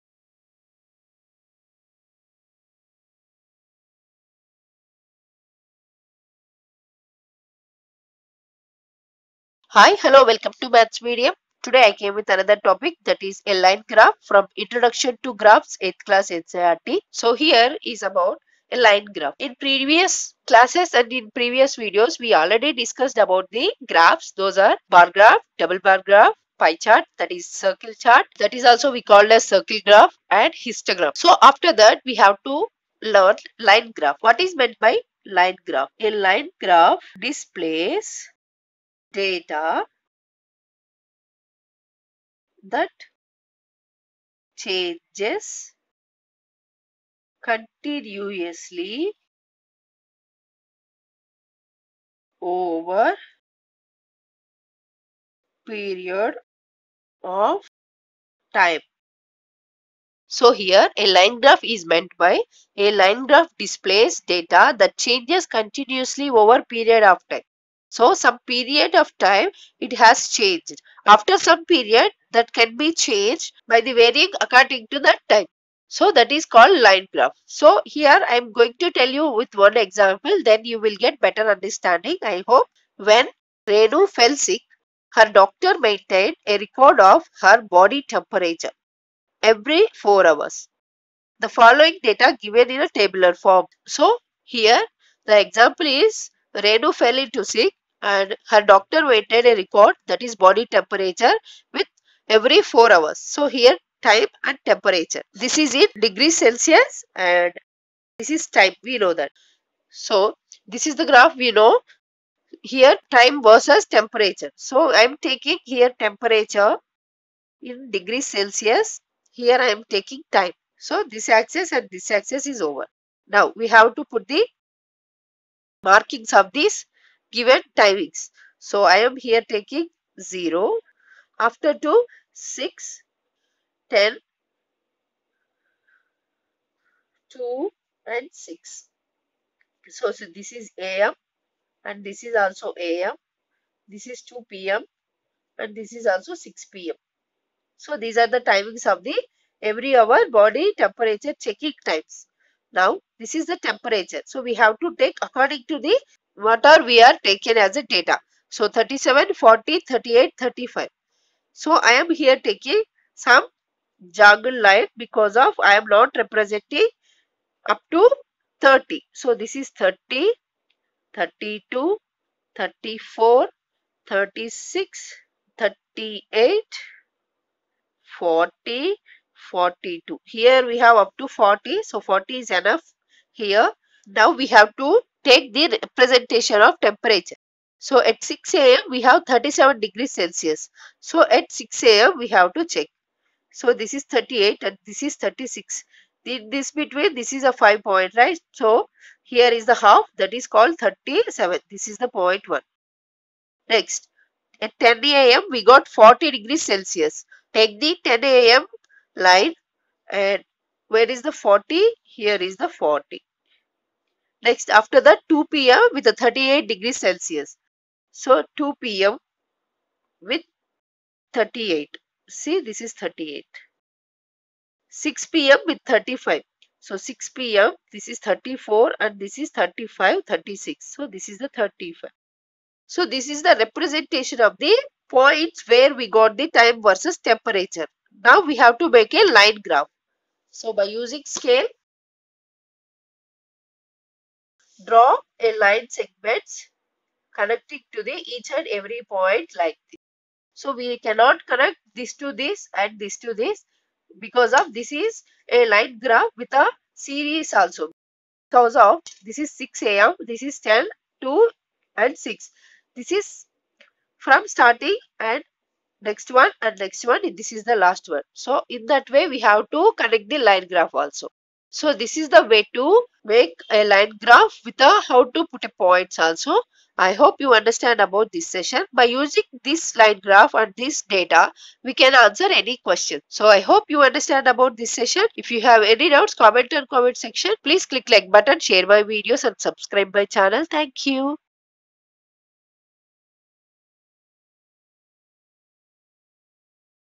A Line Graph-Class8-ncert
Example Problem:
When Renu fell sick, her doctor maintained a record of her body temperature, taken every four hours.
It is a pictorial representation of the following data, given in tabular form.
Time 6 a.m. 10 a.m. 2 p.m. 6 p.m.
Temperature(°C) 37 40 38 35

For more on Introduction to Graphs, Please Click